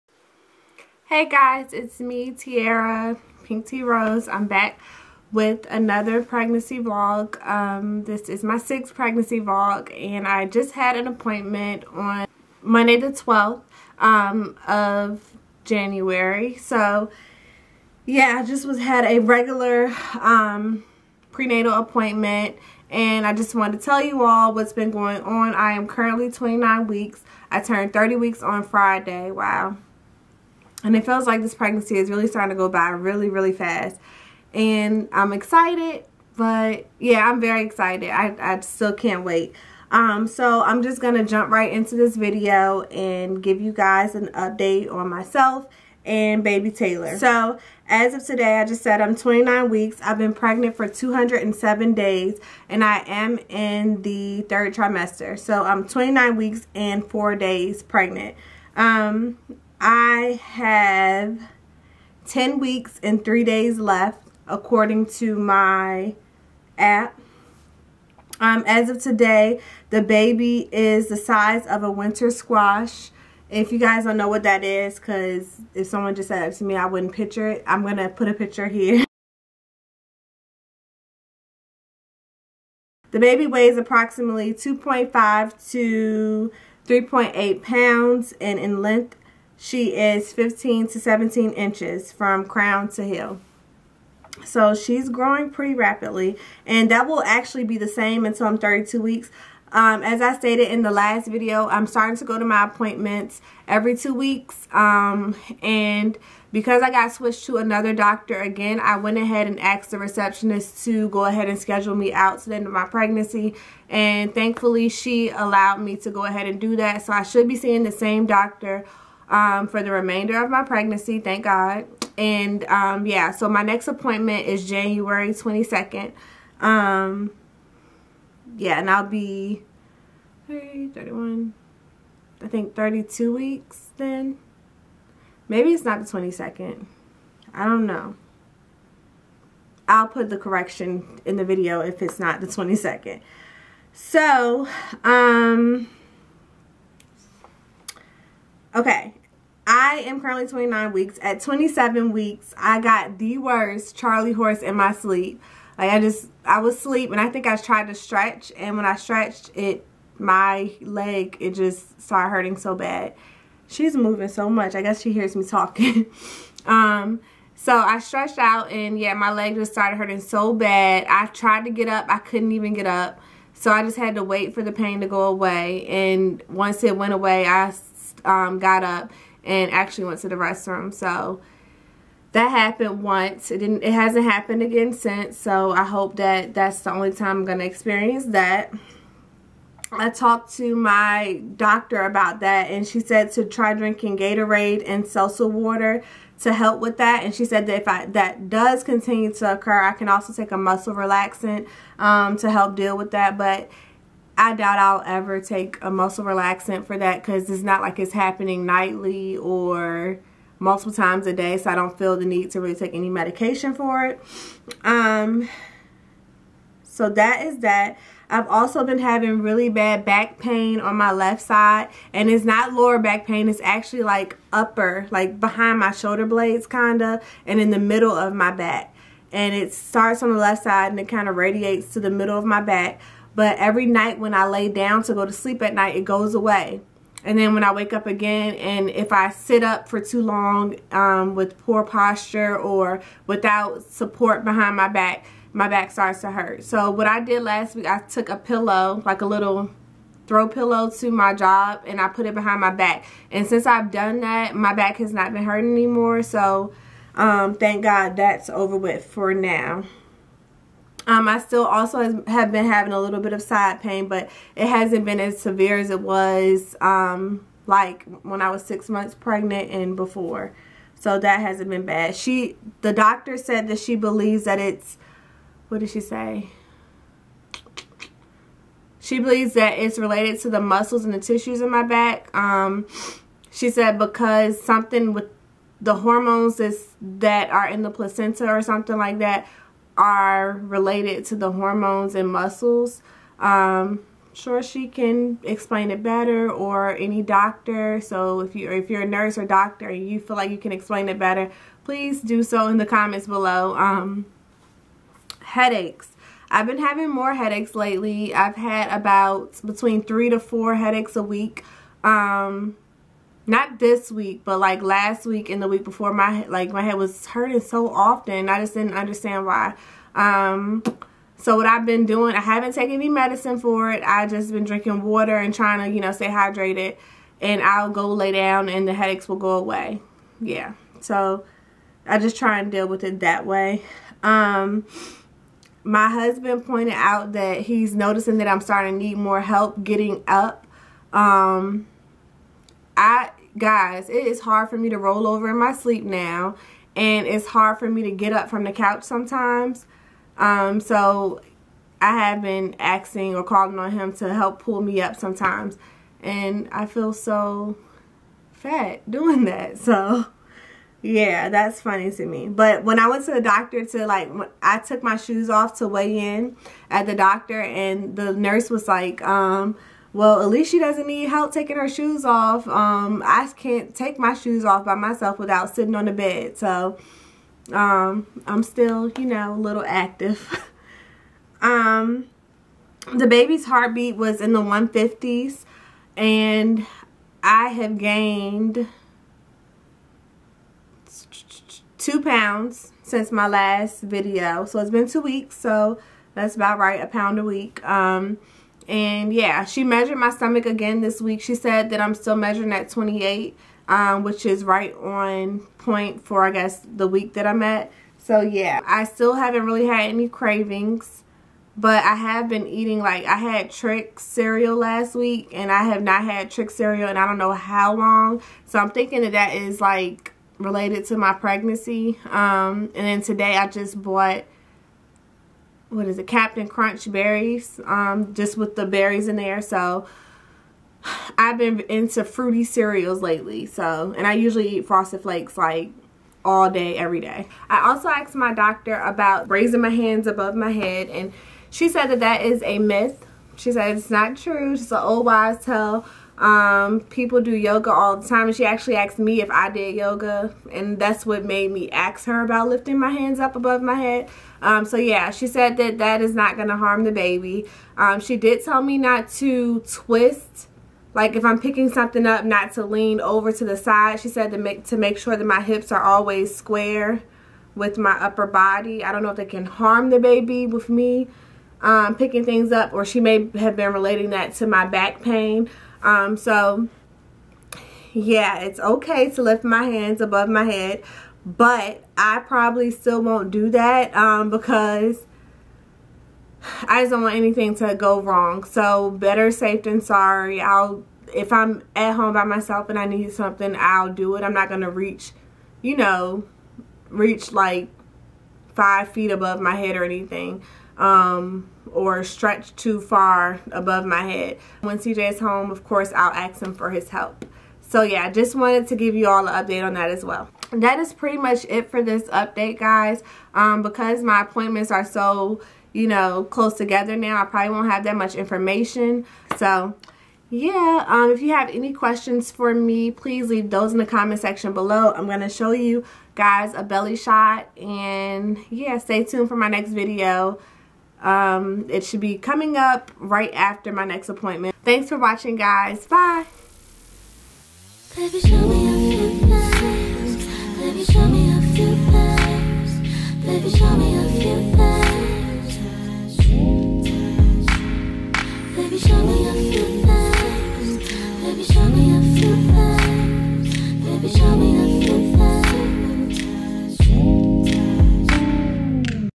Pregnancy Vlog 6- Week 27-29
Hi!!!

This blog covers weeks 27- part of week 29 (third trimester)… I turn 30 weeks Friday ;-)

My appointments are every two weeks now, so my pregnancy blogs will be closer together now.